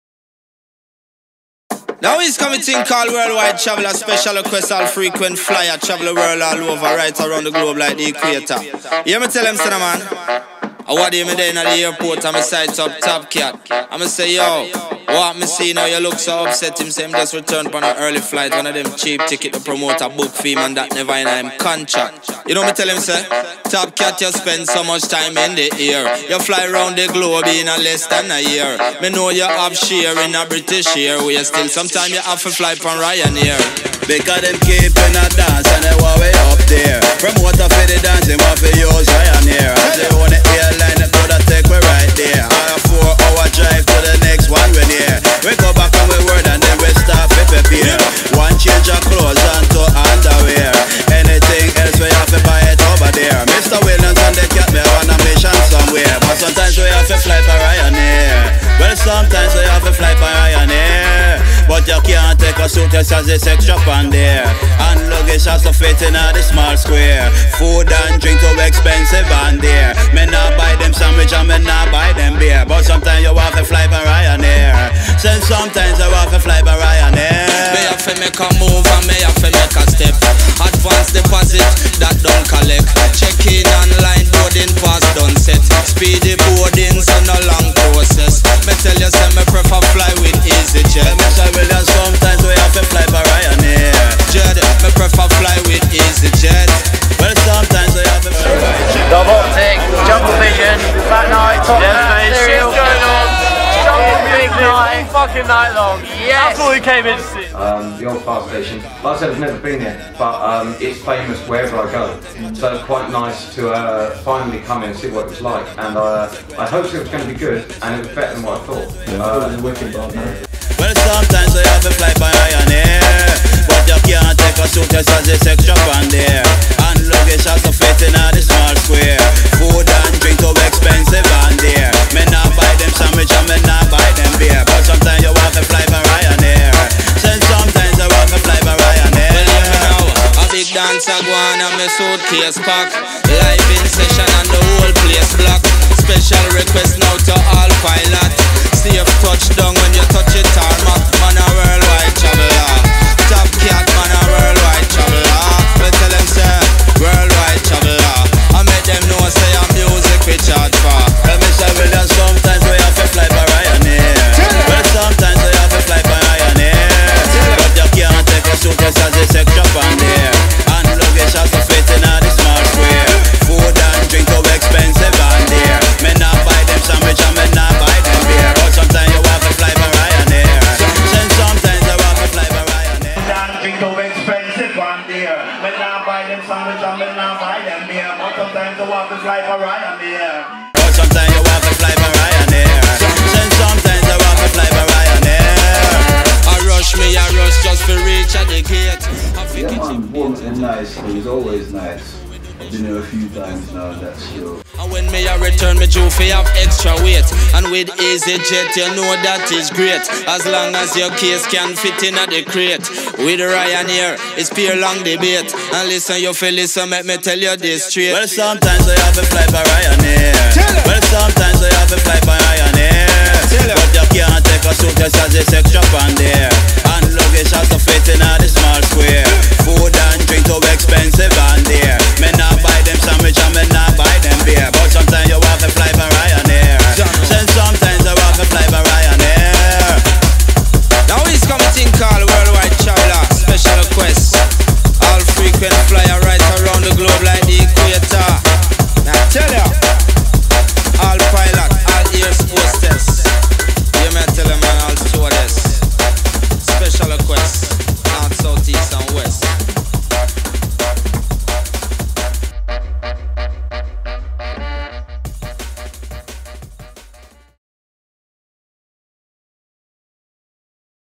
Jungle Vision, TOP CAT (live), SERIAL KILLAZ, DON CAESAR, Bournemouth Old Firestation Club
Check out our next events on APRIL 5th and May 17th now LIVE

THIS VIDEO WAS SAT MARCH 22nd, 2014 - In association with DUB OPTIC...

As you can see by the video the OLD FIRE STATION CLUB in Bournemouth was jumping all night!!!

Thanks to all performers & crew involved...

...Announcing LIVE & DIRECT, the master of ceremonies and his final show of the 'Top Cat 25th Anniversary tour!'

▇ TOP CAT ▇
& DON CEASAR
(Nine Lives Records / Congo Natty)

You know who's on the decks when you hear Rabble-rousing ragga vocals, bubbling bouncy bass, cut-throat beats & undiluted positive vibes, it's the one and only...

▇ SERIAL KILLAZ ▇
(Congo Natty / Ganja Records)

WITH SUPPORT FROM THE SOUTH'S FINEST SELECTORS

- DJ D-TOX b2b DARK DEAN

- KANAVAL SAM b2b BAMMO

- SUB-CULT

- HOST MC MANIC

AND OF COURSE...THE AWESOME & UNIQUE
- RC1 SOUNDSYSTEM !